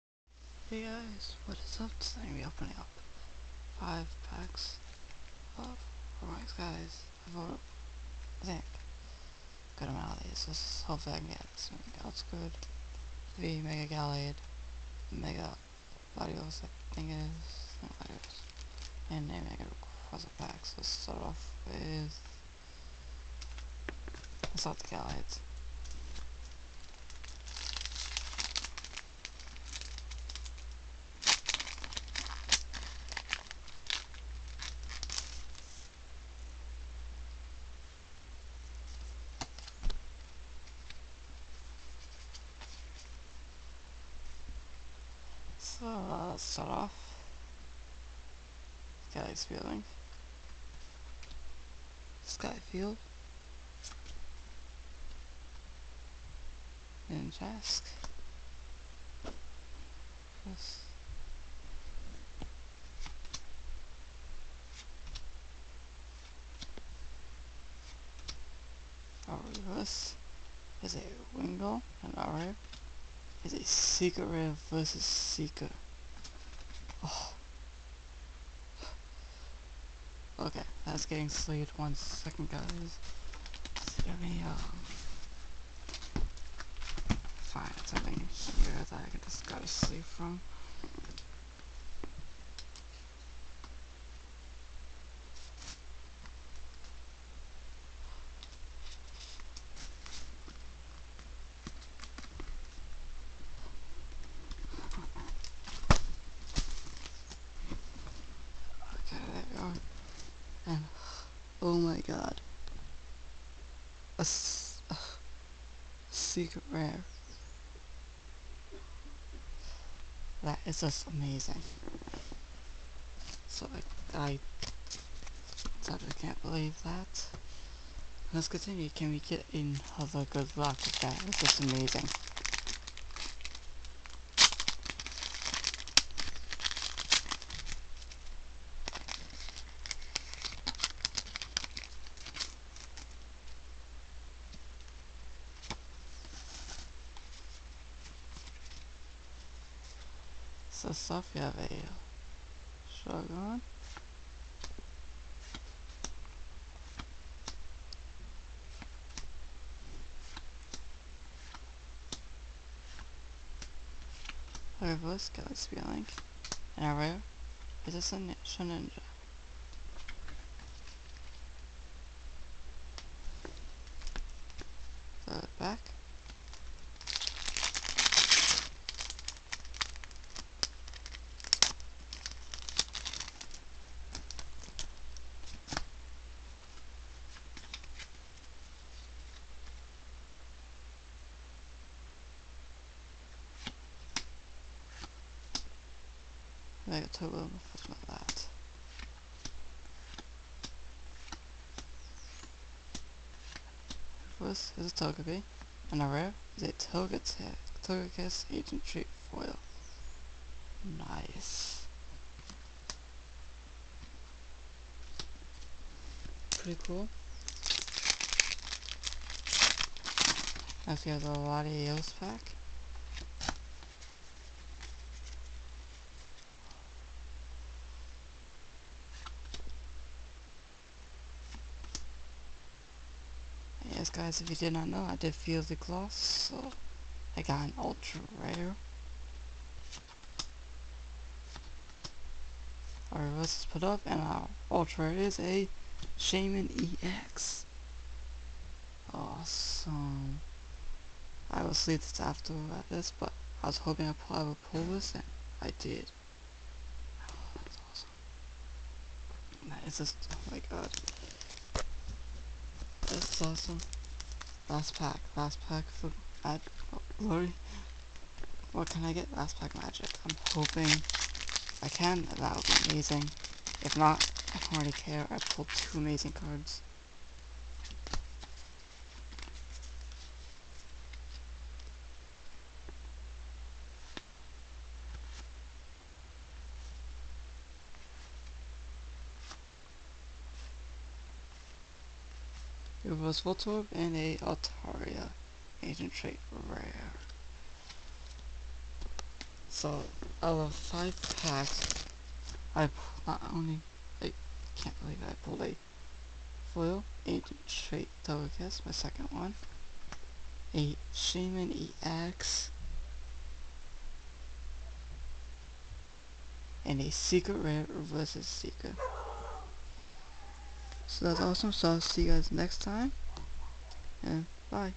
opening 5 packs of roaring skies 1ST PACK MAGIC!!
opening 5 packs of roaring skies with 1st pack magic!